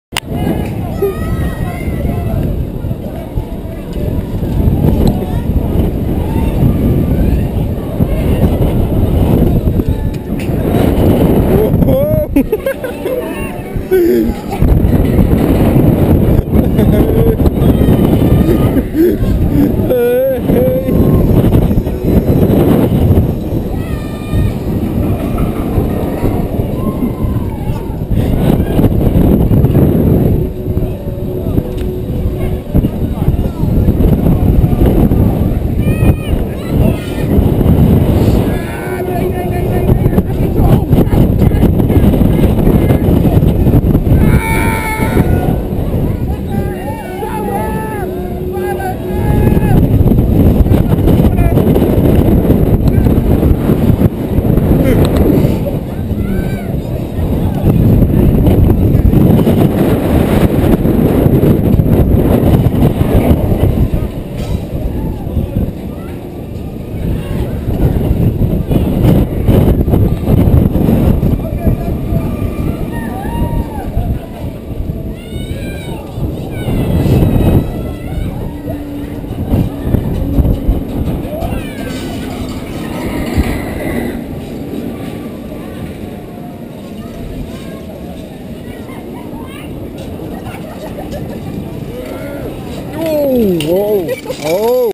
Apollo (Attractiepark Slagharen, NL)
Zwaaien, zwieren en zweven, daar gaat het om in de Apollo.
Als een vogel zo vrij, vlieg je door de lucht...
Cirkel met de attractie Apollo mee om de maan en ga op ontdekkingsreis!